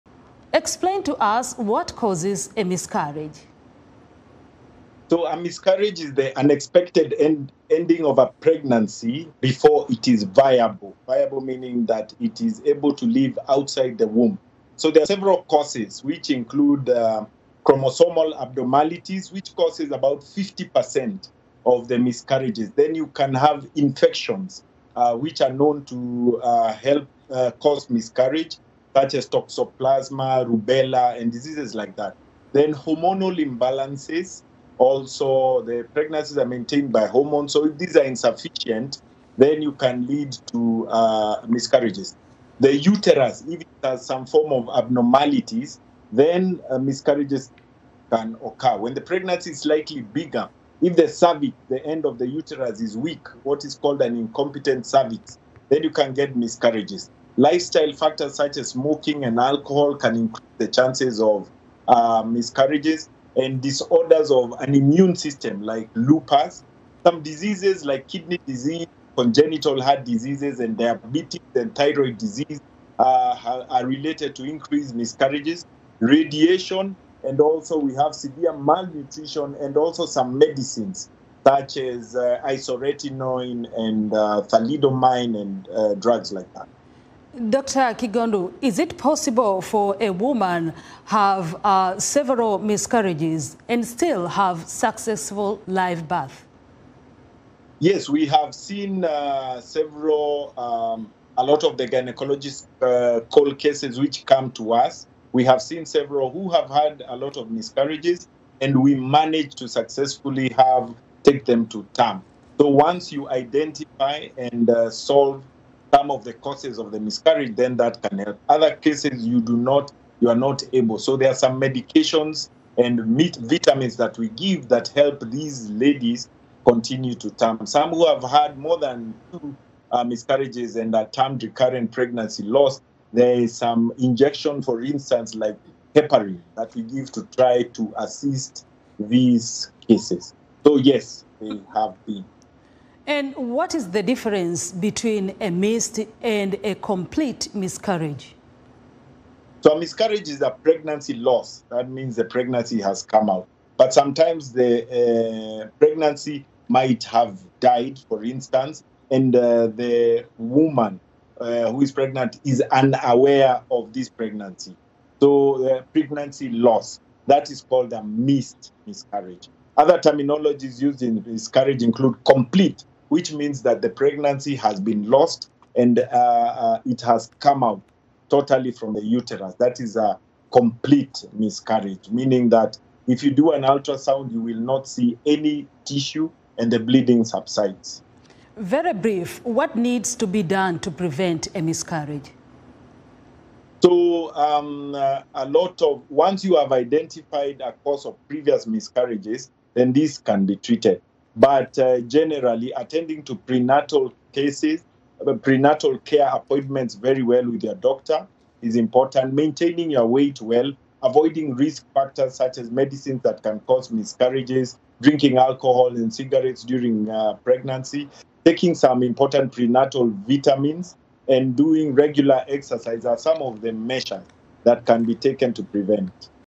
Health Report: What Causes Miscarriage?
Miscarriage is the most common reason for losing a baby during pregnancy and it is defined differently worldwide.  To understand more about what causes miscarriage and prevention, VOA health reporter Mkamiti Kibayasi spoke to Dr. Simon Kigondu, a gynecologist and CEO of Excella Healthcare at the Kenya Medical Association in Nairobi.